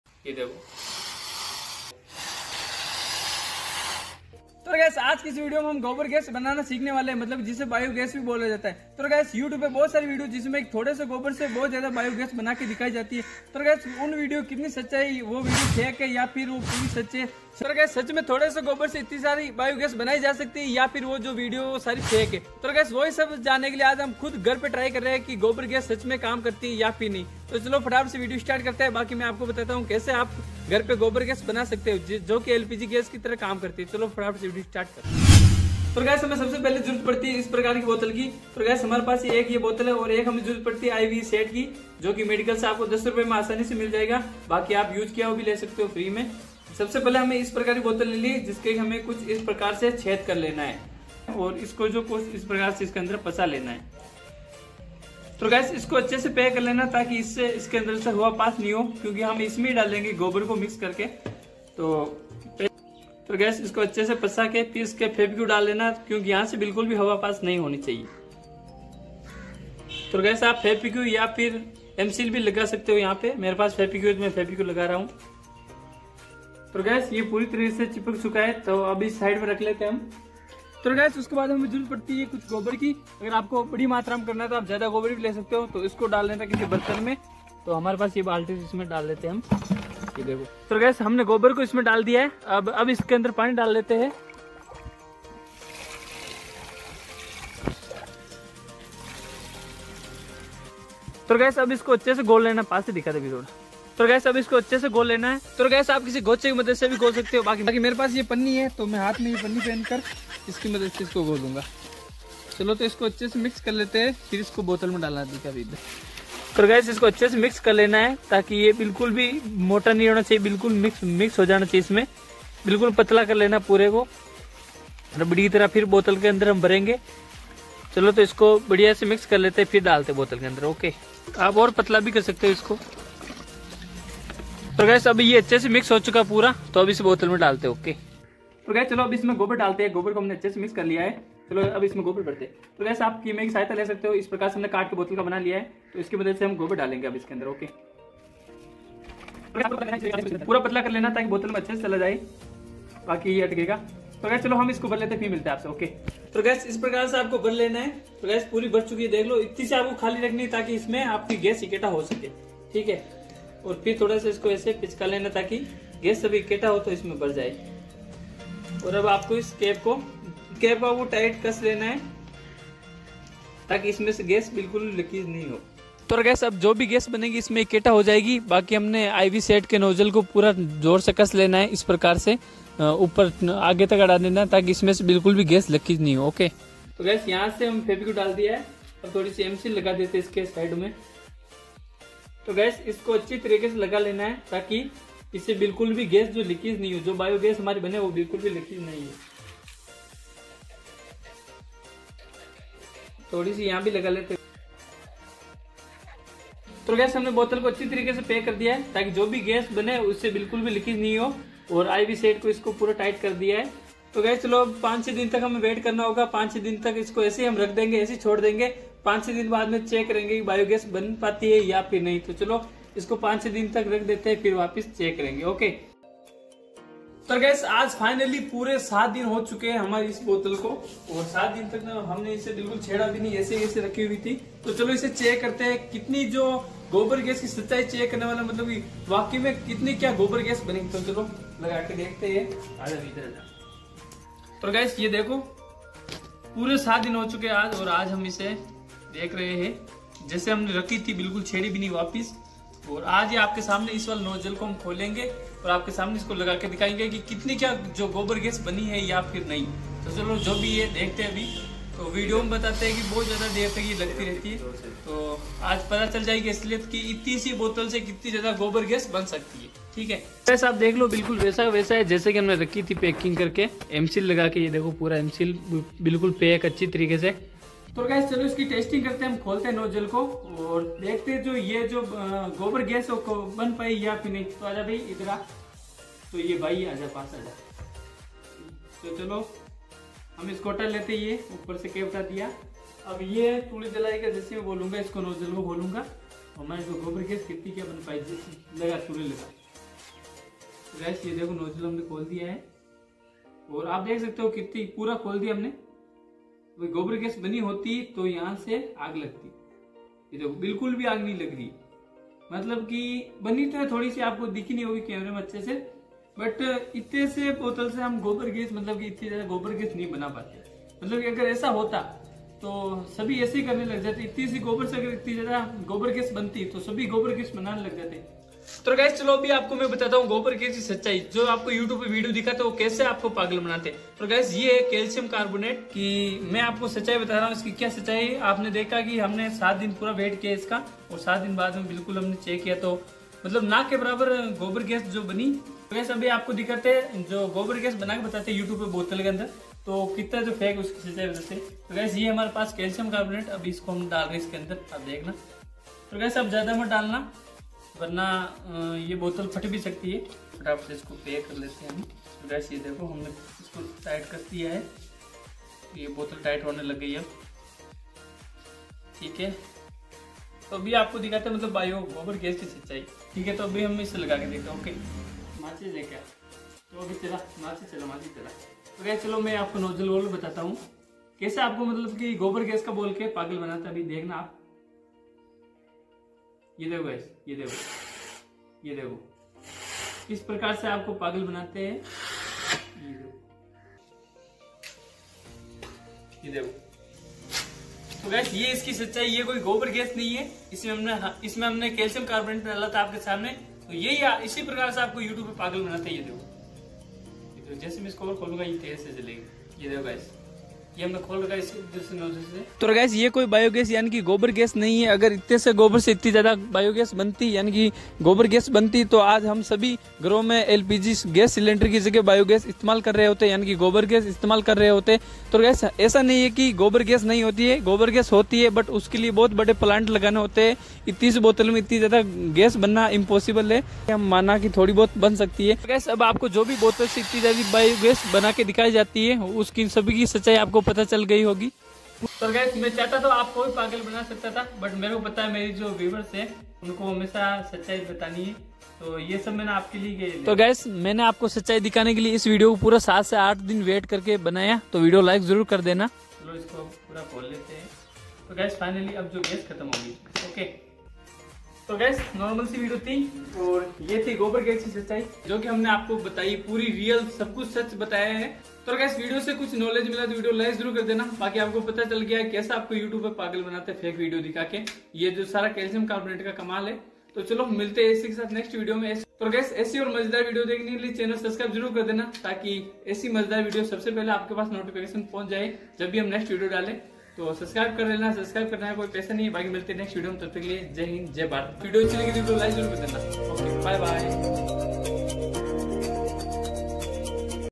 गोबर गैस की सच्चाई जान लो! Biogas Reality.जब हमने घर पर गोबर गैस बनाई तो क्यों नहीं बनी?
गोबर गैस सच्चाई जान लो! Biogas Reality.जब हमने घर पर गोबर गैस बनाई तो क्यों नहीं बनी?

How to make cow dung gas?
बायोगैस की सच्चाई!